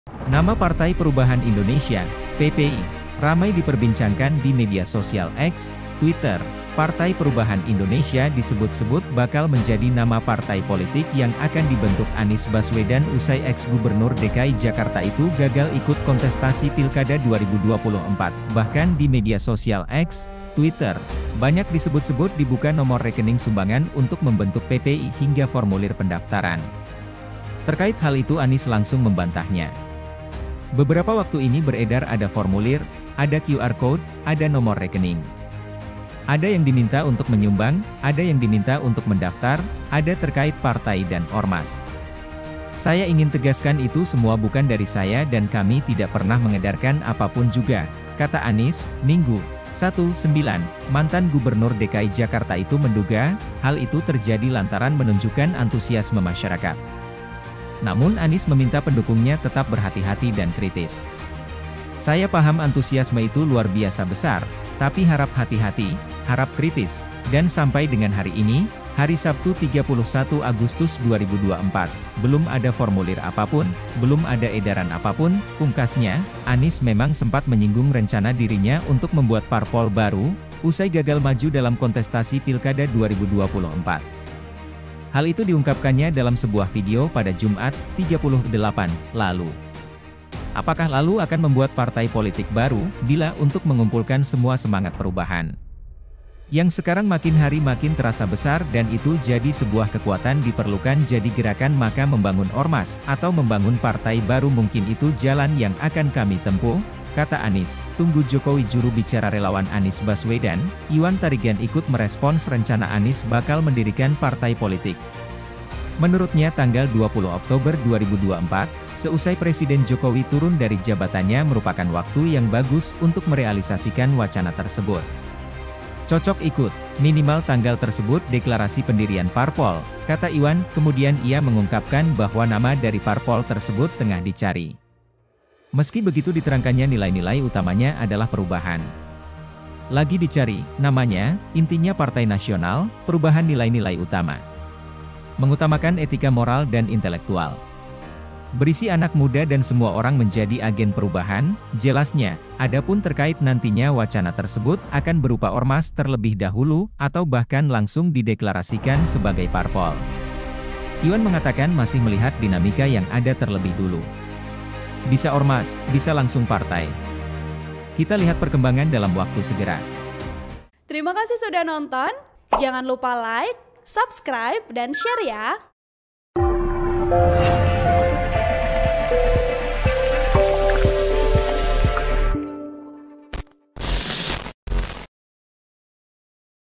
BOCORAN NAMA PARPOL BARU ANIES
Partai Perubahan Indonesia disebut-sebut bakal menjadi nama partai politik yang akan dibentuk Anies Baswedan usai eks Gubernur DKI Jakarta itu gagal ikut kontestasi pilkada 2024.

Artikel ini telah tayang di Tribunnews.com dengan judul Bocoran Nama Parpol Baru Anies, Peresmian Tunggu Jokowi Lengser,